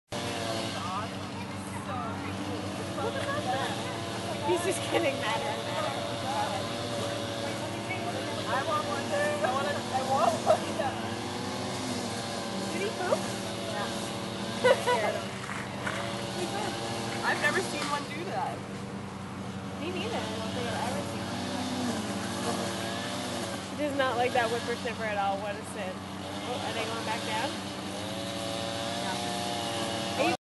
Pissed off Peacock
The peacock clearly had ill feelings towards the whipper snipper.